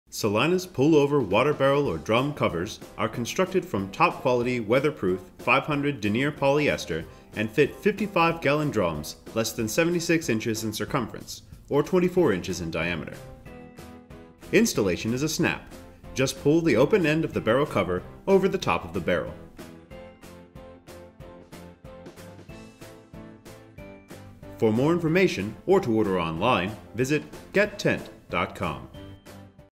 Water Barrel Cover / Drum Cover - Custom Printed
Celina's Pull-over Water Barrel or Drum Covers are constructed from top quality weatherproof  500 Denier [den-ear] polyester and fit 55 gallon drums less than 76 inches in circumference, or 24 inches in diameter.

Questions?